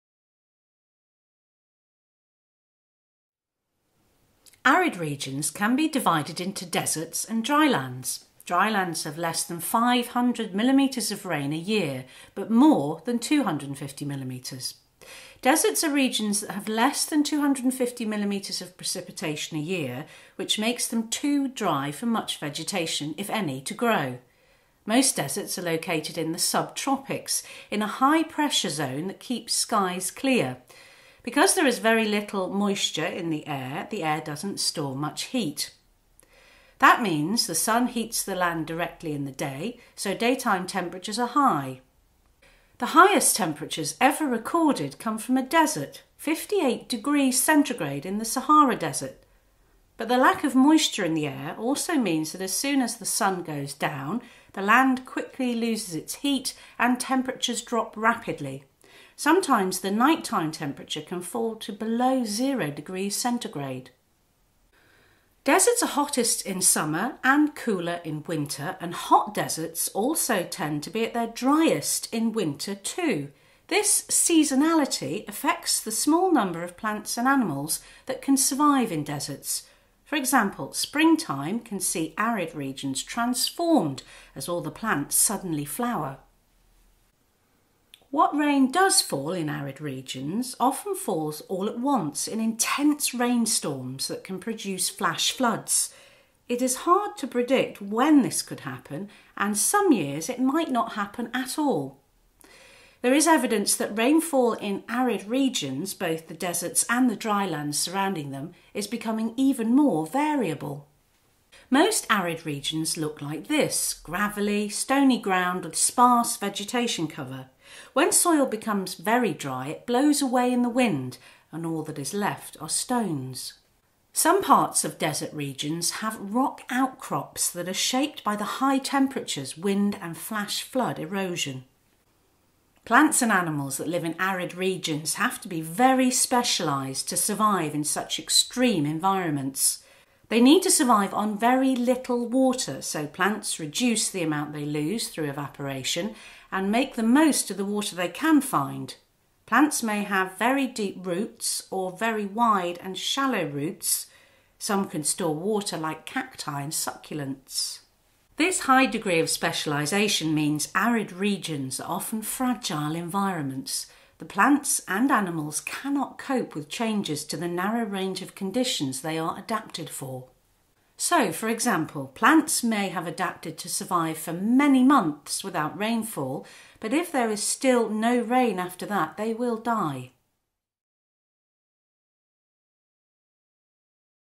Edexcel GCSE Geography - Features of Arid Regions
Edexcel GCSE Geography A and B Unit 1

This video explains how arid regions are divided into deserts and dry lands.